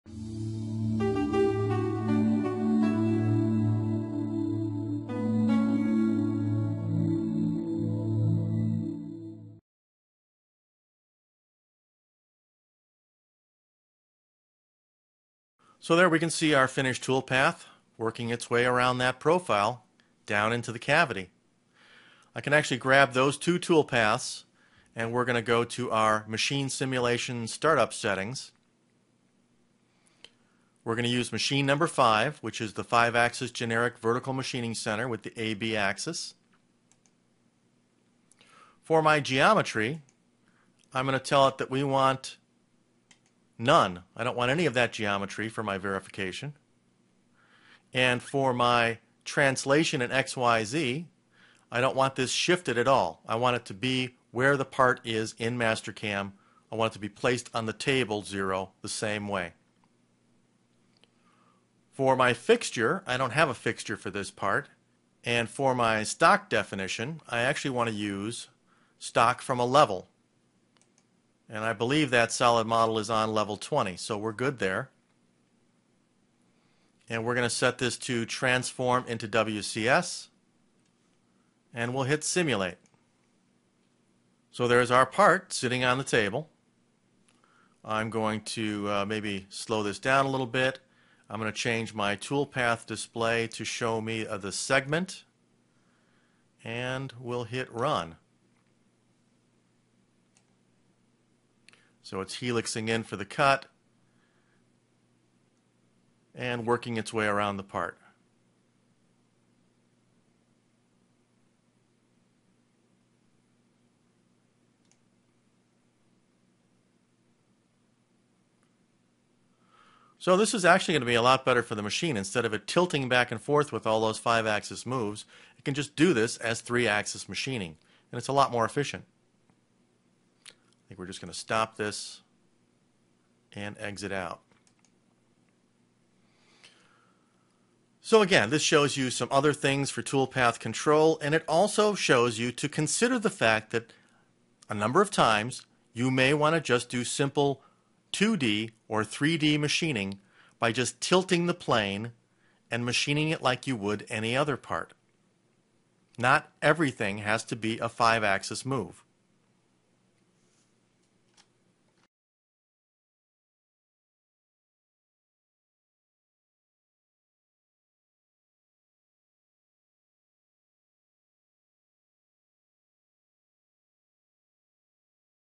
Mastercam 5X Control - 3+2 Machining Video 4 of 4
This video series talks about doing 3 axis surface machining on a 5 axis machine (2x positioning and 3x cutting). It was supposed to be on the X5 MultiAxis Disk #2, but just didn't fit. This is Video #4 in a series of 4 that will show, setting the Plane, Defining The Stock, Roughing The Cavity, Finishing The Cavity and Machine Simulation.

Be sure to click on "Large Player" and "Quality" of 480 when watching this video.